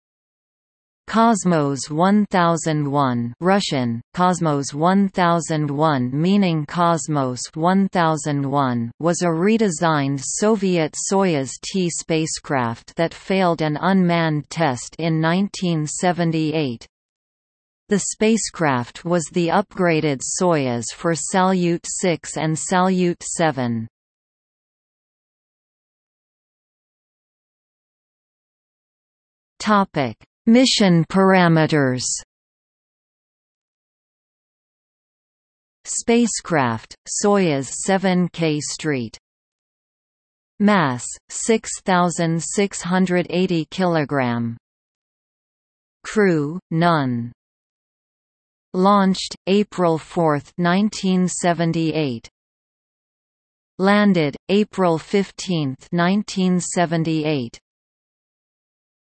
Kosmos 1001 | Wikipedia audio article
00:00:26 Mission parameters

- Socrates

SUMMARY
Kosmos 1001 (Russian: Космос 1001 meaning Cosmos 1001) was a redesigned Soviet Soyuz T spacecraft that failed an unmanned test in 1978.  The spacecraft was the upgraded Soyuz for Salyut-6 and Salyut-7.